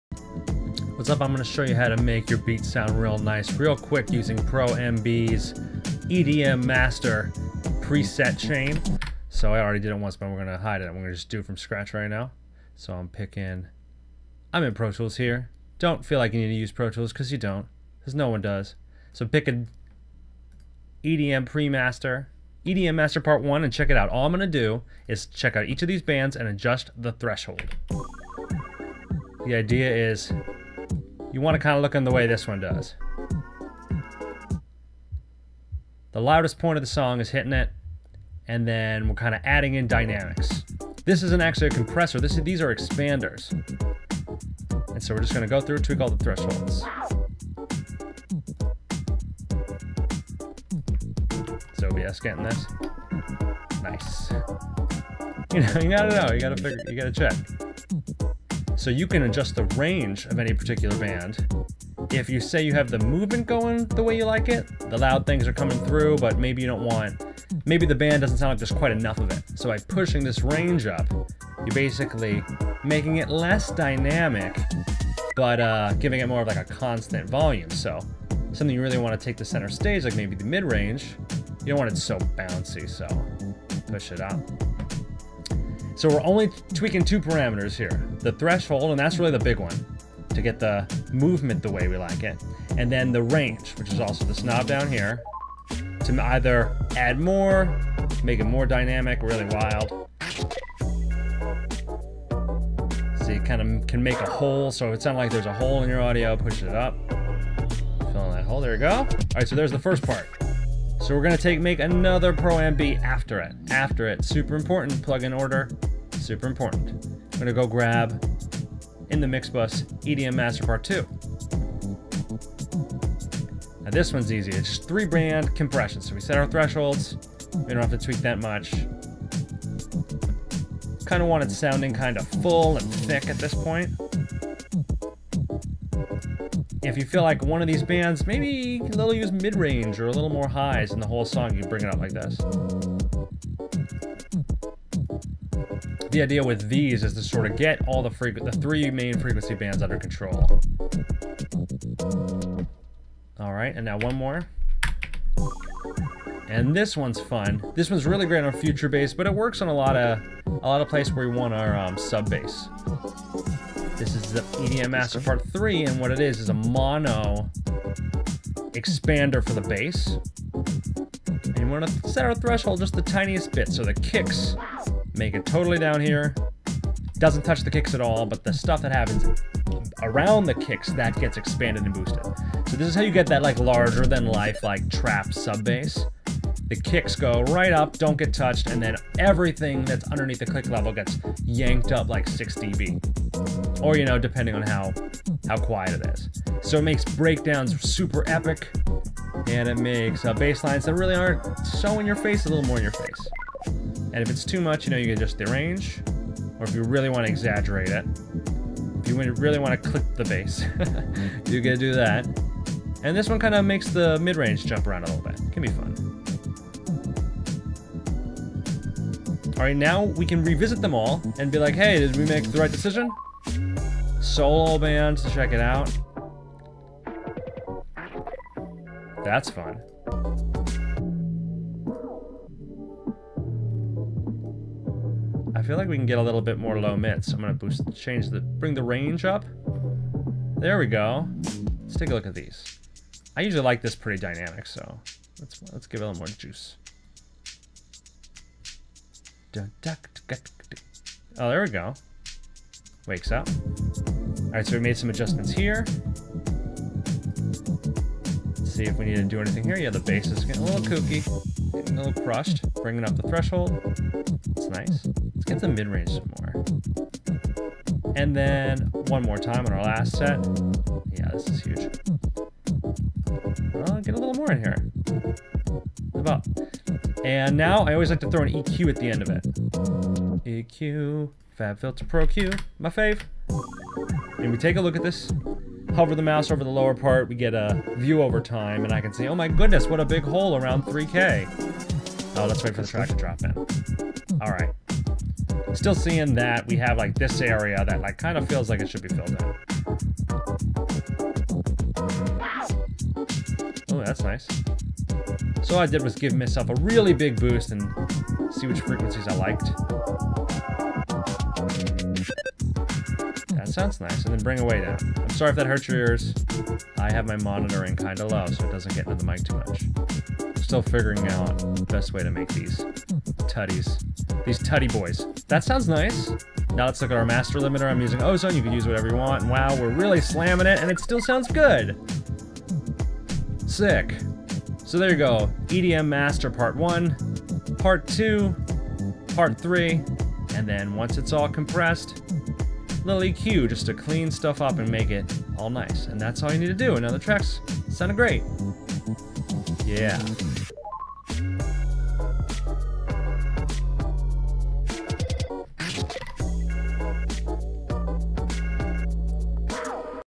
How to Master with FabFilter Pro-MB (EDM Master Chain)
Here's a quick example of how I use the three stage "EDM Master" preset chain that comes with FabFilter Pro-MB. The first instance is a five band expander, the second is a three band compressor, adn finally, a dual band expander. Then i use FabFilter Pro-Q 2, an EQ plugin, to round it out before a master limiter.